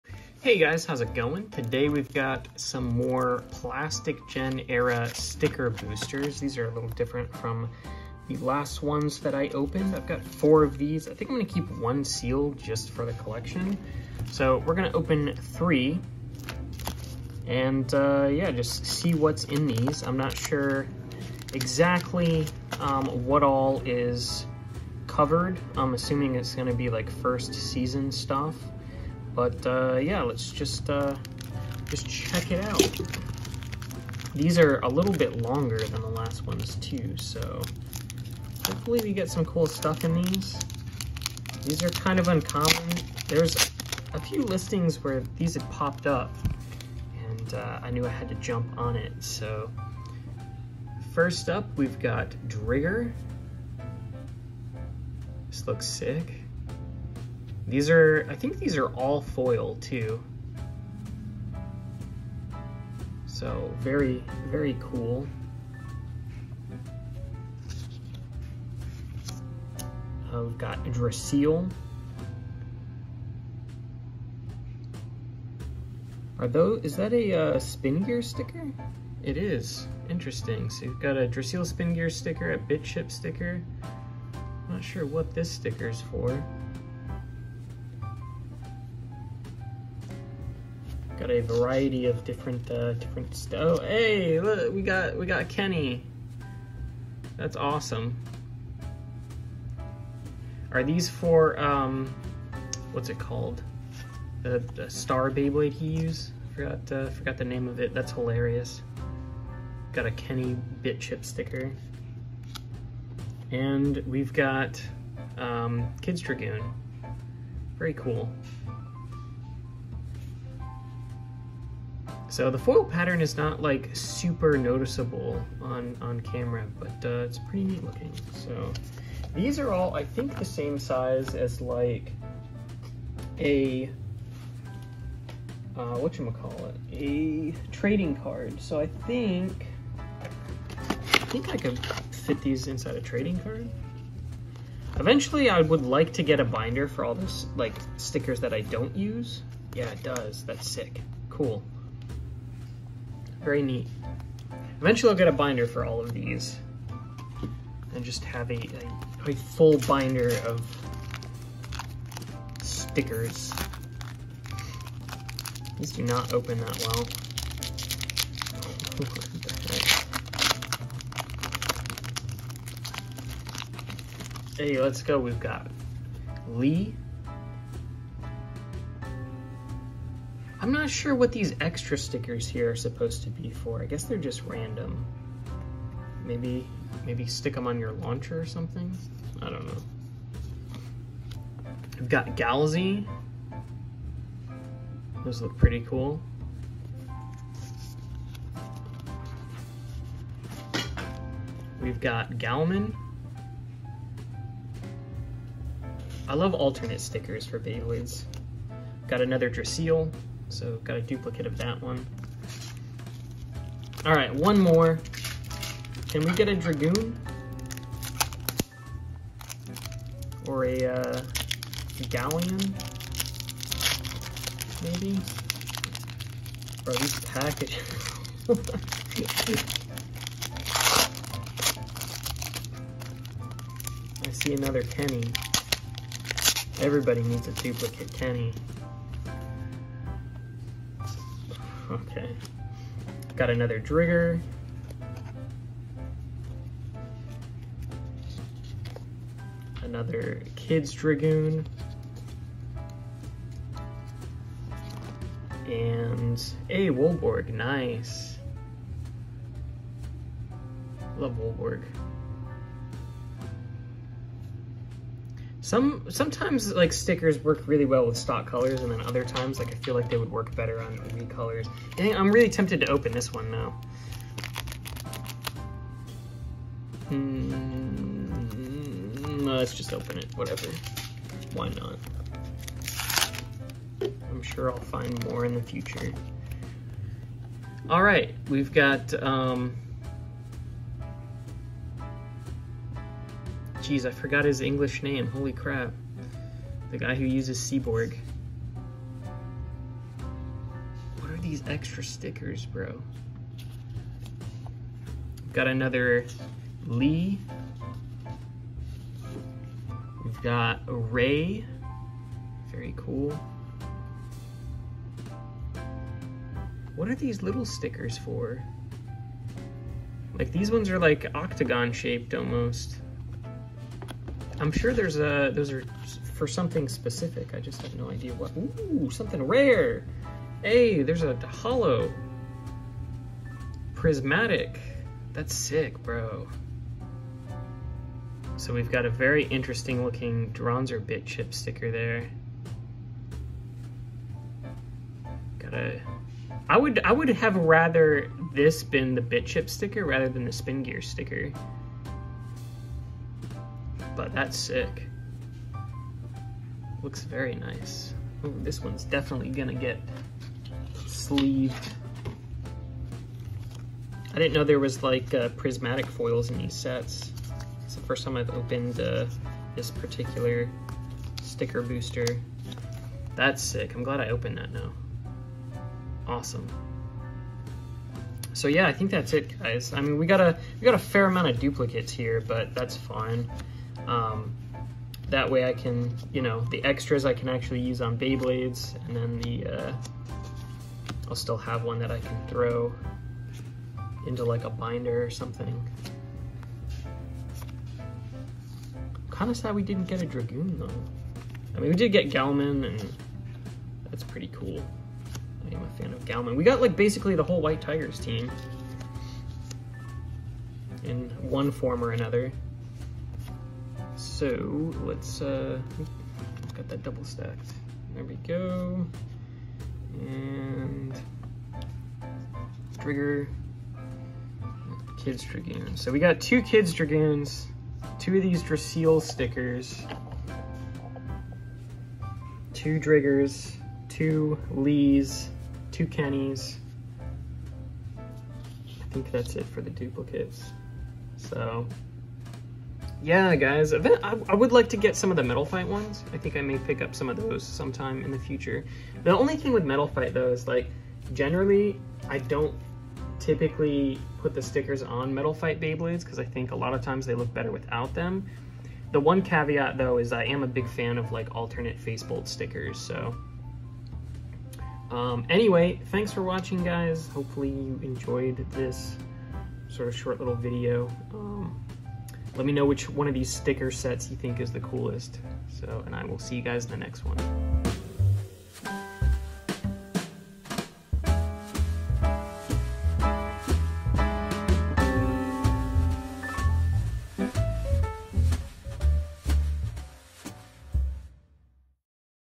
Opening Rare Bakuten Shoot Beyblade Custom Stickers
Just opening some pretty rare bakuten shoot beyblade sticker packs. We get some pretty cool stuff out of these.